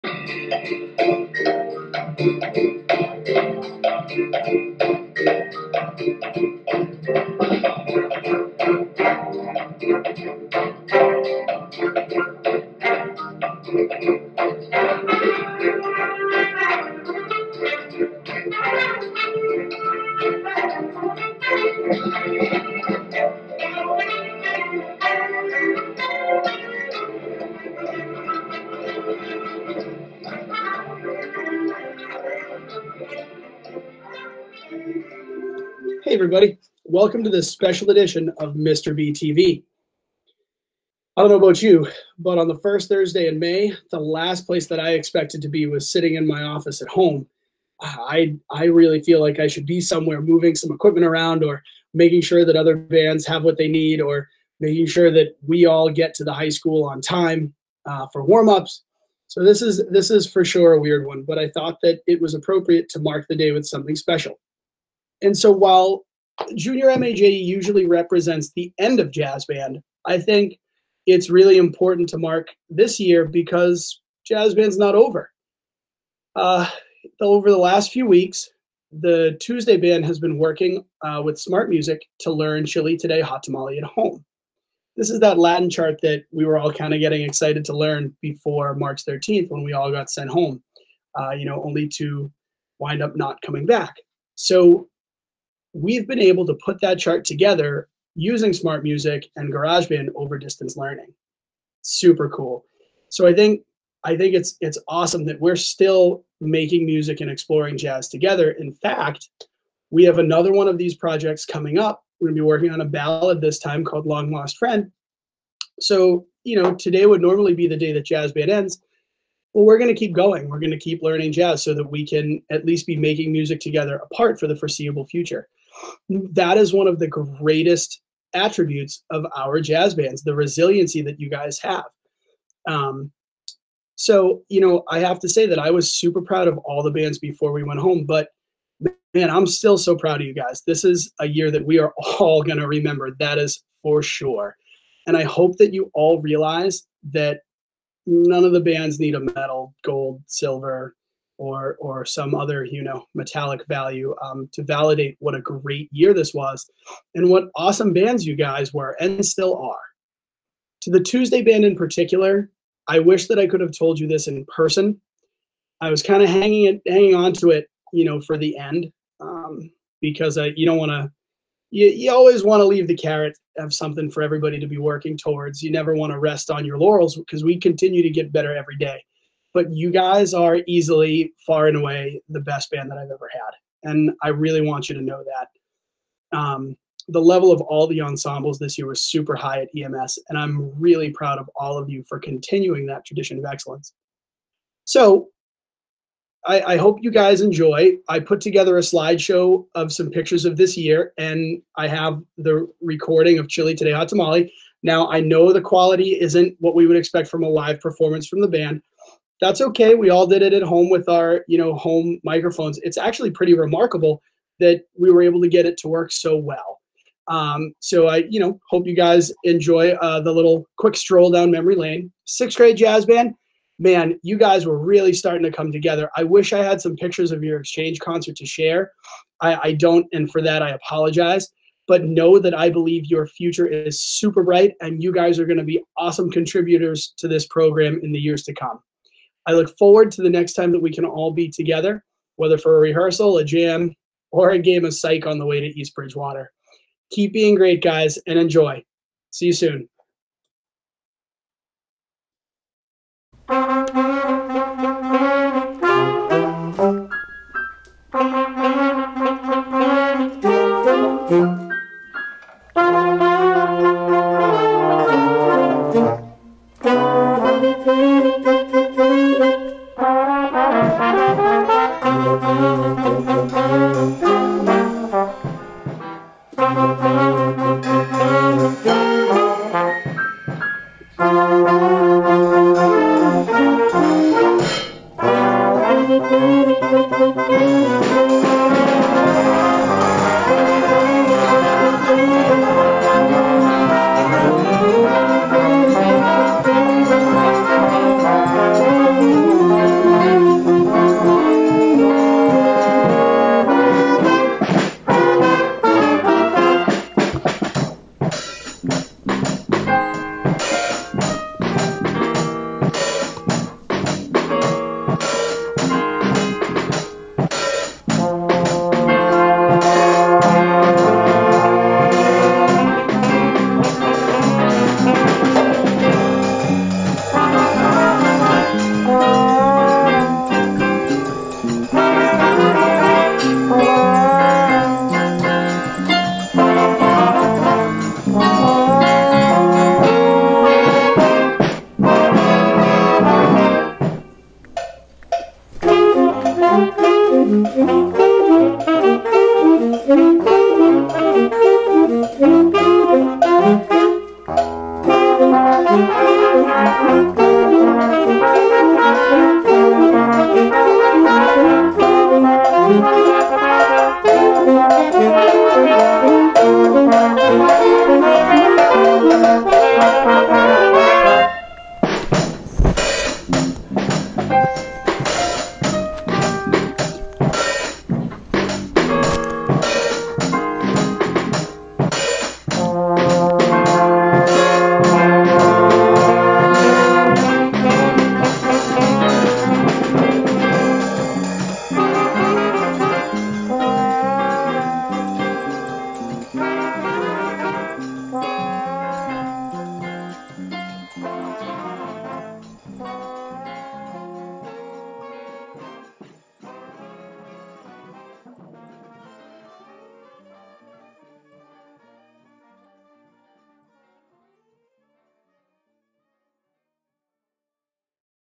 Mr B TV Special Jazz Band Edition
A special Jazz Band episode to mark the day that would have been Jr. MAJE with a special performance!